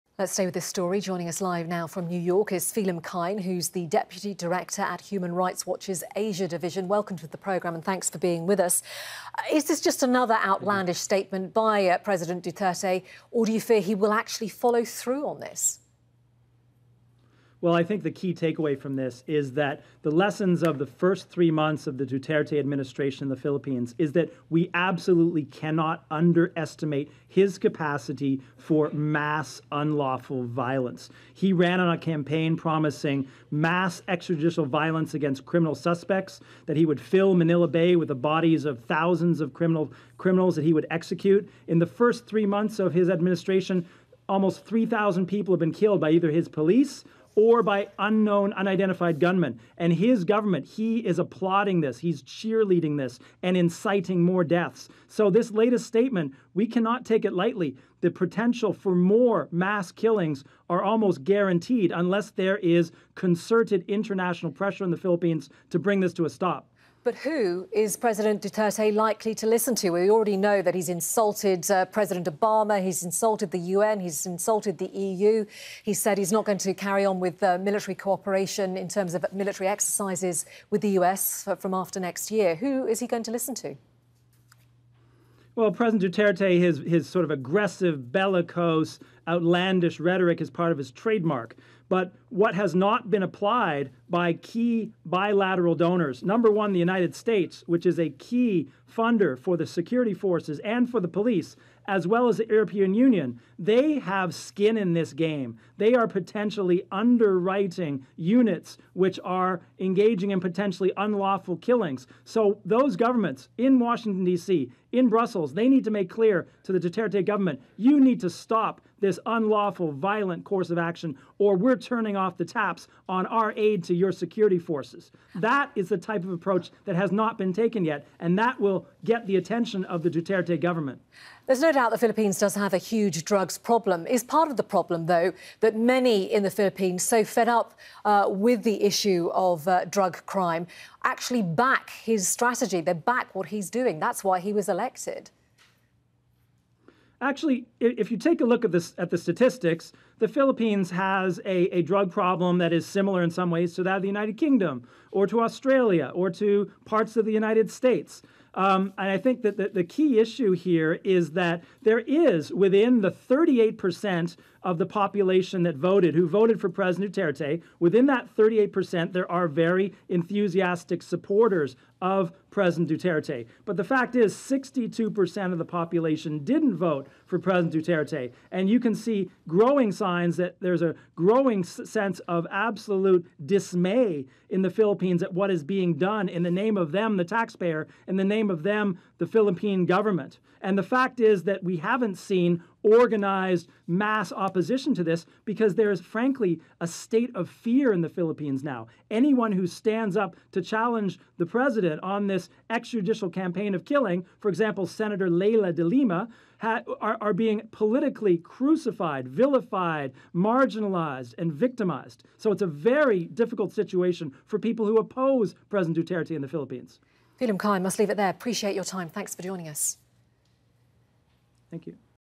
Human Rights Watch: Duterte inciting more deaths in the Philippines
Phelim Kine, deputy director for the Asia division of Human Rights Watch, said that the Philippine President Rodrigo Duterte's capacity for "mass unlawful violence" should not be underestimated.

Kine urged the United States and the European Union to put more pressure on the Duterte administration to put a stop on the killings.